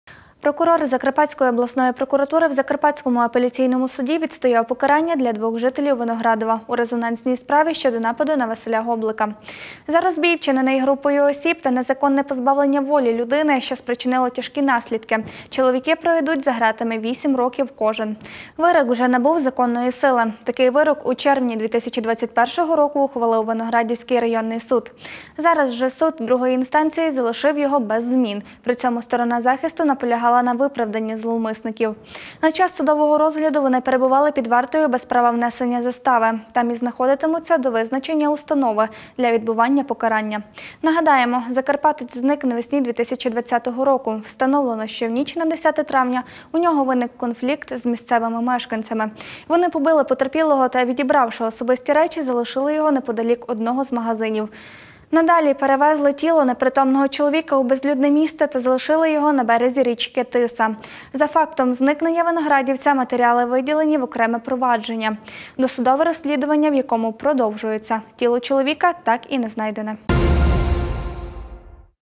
Резонансна справа. Покарання для винних
Прокурор Закарпатської обласної прокуратури в Закарпатському апеляційному суді відстояв покарання для двох жителів Виноградова у резонансній справі щодо нападу на Василя Гоблика. За розбій, вчинений групою осіб (ч. 2 ст. 187 КК України) та незаконне позбавлення волі людини, що спричинило тяжкі наслідки (ч. 3 ст. 146 КК України) чоловіки проведуть за гратами 8 років кожен. Вирок вже набув законної сили. Такий вирок у червні 2021 року ухвалив Виноградівський районний суд. Зараз же суд другої інстанції залишив його без змін, при цьому сторона захисту наполягала на виправданні зловмисників. На час судового розгляду вони перебували під вартою без права внесення застави, там і знаходитимуться до визначення установи для відбування покарання. Нагадаємо, закарпатець зник навесні 2020 року. Встановлено, що в ніч на 10 травня у нього виник конфлікт з місцевими мешканцями. Вони побили потерпілого та, відібравши особисті речі, залишили його неподалік одного з магазинів. Надалі перевезли тіло непритомного чоловіка у безлюдне місце та залишили на березі річки Тиса. За фактом зникнення виноградівця матеріали виділені в окреме провадження за ч. 1 ст. 115 КК України, досудове розслідування у якому продовжується. Тіло чоловіка ще так і не знайдене.